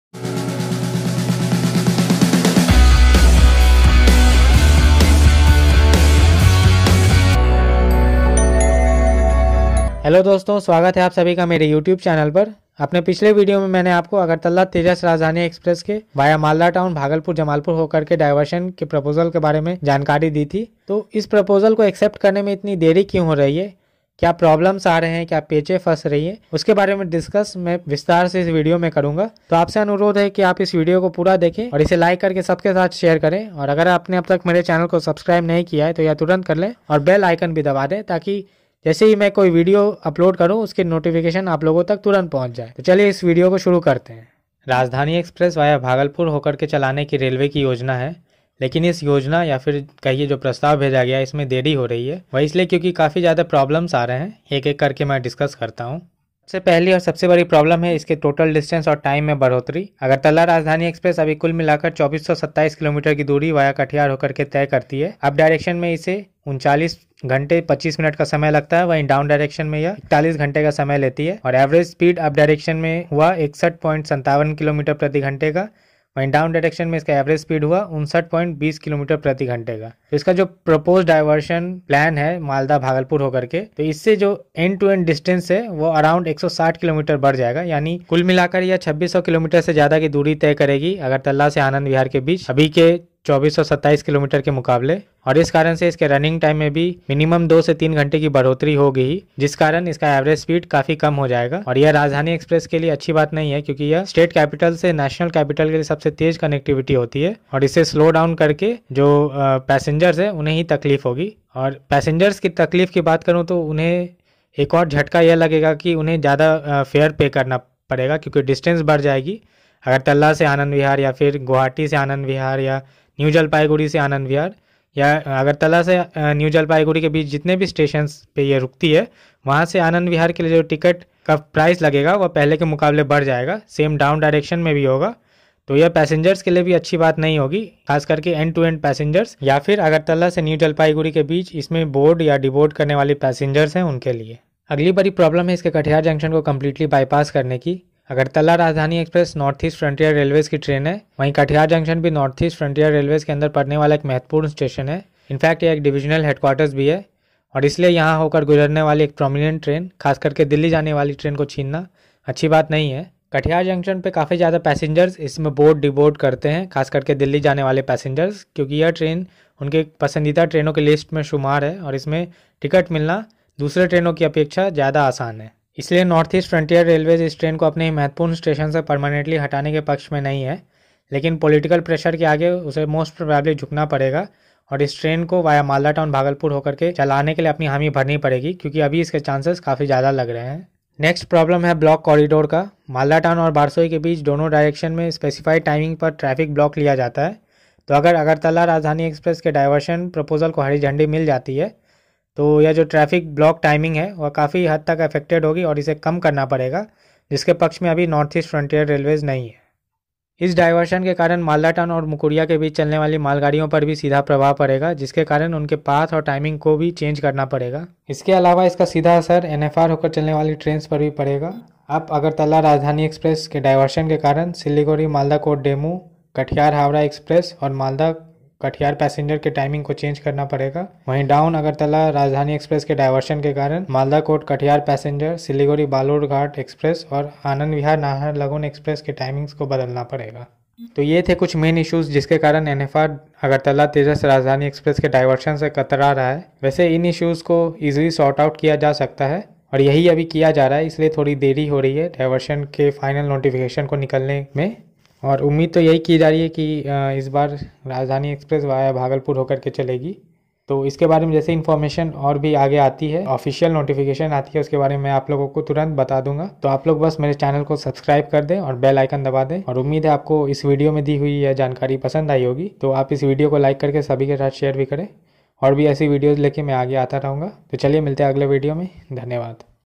अगरतला राजधानी वाया भागलपुर चलाने में देरी क्यूँ?। Agartala Rajdhani Express Diversion Via Bhagalpur
अगरतला राजधानी वाया भागलपुर चलाने में देरी क्यूँ?
Agartala Rajdhani Express Diversion Via Bhagalpur - Complications In Proposal Explained

In This Video, I Have Discussed The Issues And Complications Faced By Railways In The Diversion Proposal Of Agartala Tejas Rajdhani Express Via Malda Town, Sahibganj, Bhagalpur, Jamalpur.

For Full Details Of Diversion Proposal of Agartala Tejas Rajdhani Express, Click The Link Below-

Hope You'll like the information shared in the video and share it with others.

PS- I've Used The Video Patna Tejas Rajdhani Express In The Backdrop.

For More Similar Videos, Click The Links Below-

About Agartala Rajdhani Express
20501 / 20502 Agartala Rajdhani Express is a Rajdhani class train belonging to the Northeast Frontier Railway zone, and runs between Agartala and Anand Vihar Terminal in New Delhi, India. It is currently being operated as train numbers 20501/20502 on a weekly basis.
It Is The First Rajdhani Express Of Indian Railways to Run With Advanced Tejas Express Coaches.

20501 Agartala – Anand Vihar Terminal Rajdhani Express has an average speed of 59 km/h and covers 2422 km in 40 h 50 min.

The 20502 Anand Vihar Terminal – Agartala Rajdhani Express has an average speed of 58 km/h and covers 2422 km in 41 h 40 min.

The Train Stops At These Stations:
Agartala
Ambassa
Dharmanagar
New Karimganj Junction
Badarpur Junction
New Haflong
Hojai
Guwahati
Kamakhya Junction
Rangiya Junction
Barpeta Road
New Jalpaiguri Junction
Katihar Junction
Barauni Junction
Patna Junction
Mughalsarai Junction
Kanpur Central
Anand Vihar Terminal

The train has standard LHB rake of 16 coaches, capable of achieving a maximum speed of 130 km/hr. The coach configuration is:
1 First AC
2 AC II Tier
10 AC III Tier
1 Pantry Car
2 End-on Generator Car

A Siliguri based WDP 4/4B/4D locomotive hauls the train from Agartala to Katihar, and from Katihar to Anand Vihar Terminal, the train is hauled by a Ghaziabad based WAP 7 locomotive & vice versa.

Train No. 20501 Agartala to Anand Vihar Terminal Rajdhani Express runs on Monday from Agartala and reaches Anand Vihar Terminal on Wednesday. Train No. 20502 Anand Vihar Terminal to Agartala Rajdhani Express runs on Wednesday from Anand Vihar Terminal and reaches Agartala on Friday.